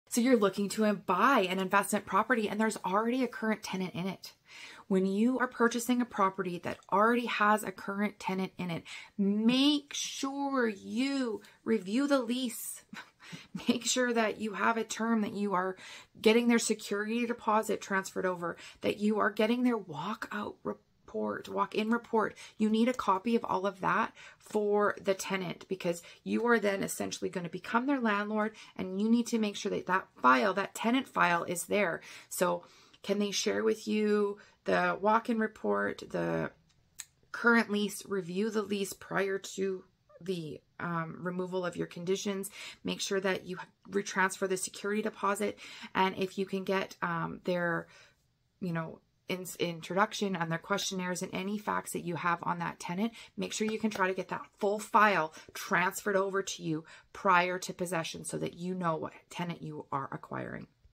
📈What to know when buying an investment property that already has a tenant
📈When buying an investment property that already has a tenant, it is important to properly review the lease agreement, and transfer the security deposit prior to possession.

 👉Additionally, make sure that the tenant file is transferred, including the walk in report, current lease, introduction, questionnaire, and other pertinent information.

 👉This will help ensure that the buyer is properly prepared to become the landlord of the tenant.